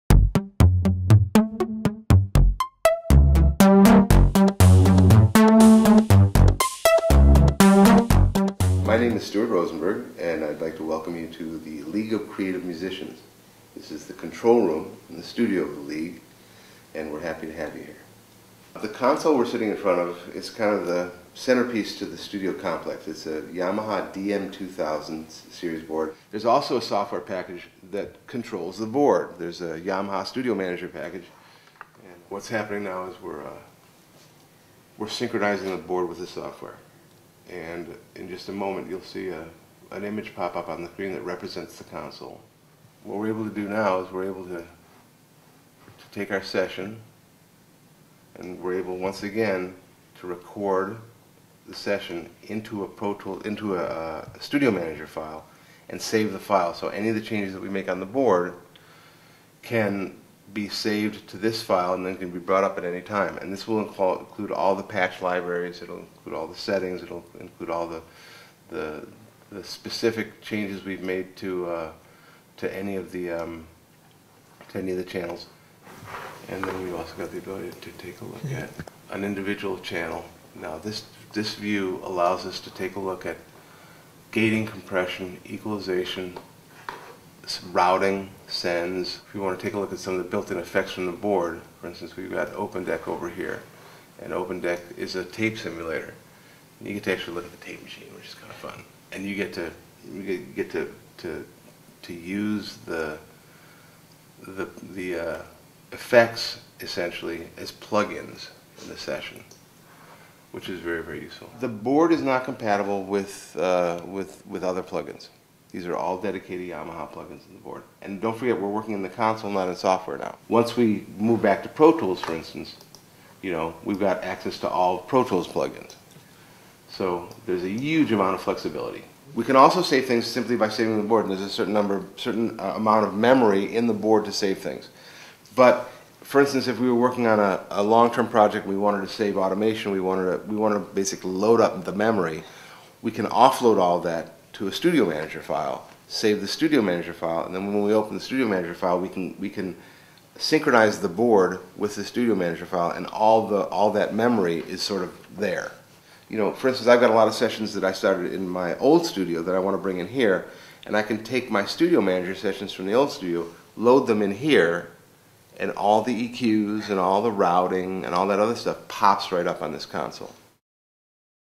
Yamaha Studio Manager Software At The Leage Of Creative Musicians
I remember how excited I was to play the VHS driven board game Nightmare when I was a kid. Yamaha Studio Manager is like the same thing as the VHS for the DM2000 board without all the shame.